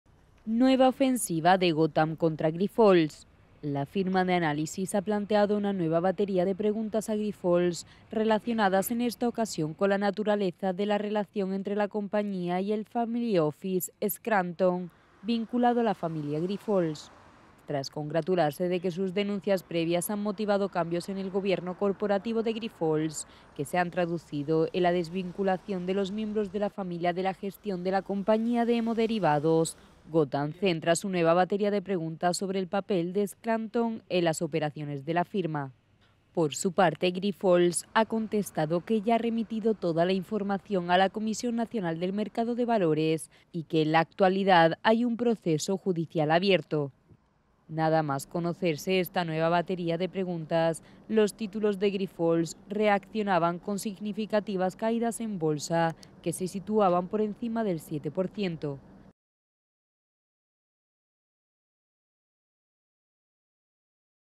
Las acciones de Grifols se desploman un 7 % tras un nuevo ataque bajista de Gotham
La firma de análisis Gotham City ha planteado una nueva batería de preguntas a Grifols relacionadas en esta ocasión con la naturaleza de relación entre la compañía y el 'family office' Scranton, vinculado a la familia Grifols.